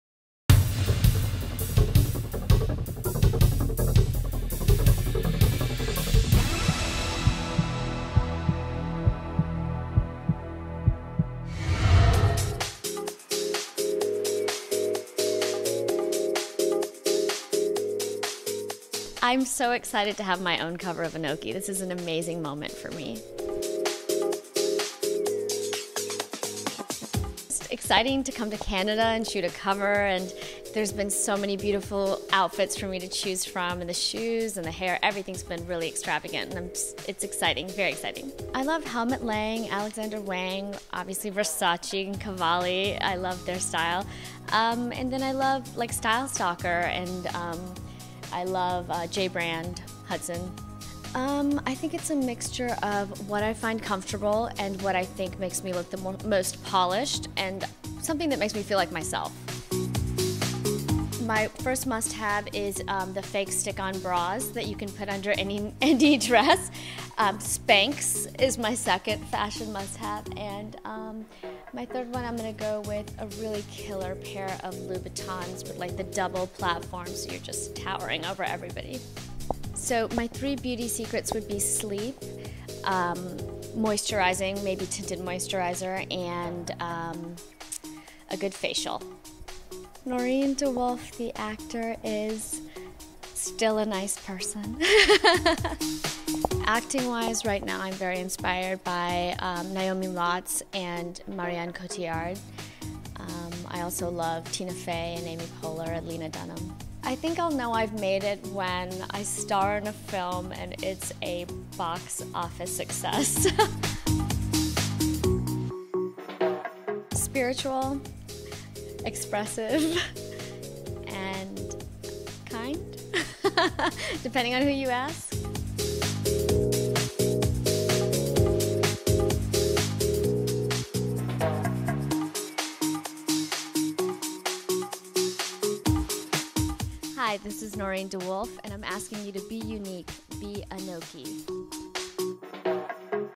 APTV: ANOKHI Magazine's Summer 2013 Health & Wellness Issue Cover Shoot With Noureen Dewulf
Catch the behind-the-scenes cover shoot of 'Anger Management' (also starring Charlie Sheen) actress, Noureen DeWulf for ANOKHI Magazine's Summer 2013 Health & Wellness issue. See what this primetime TV star has to say about her fashion & style likes, must have wardrobe accessories, beauty secrets and so much more.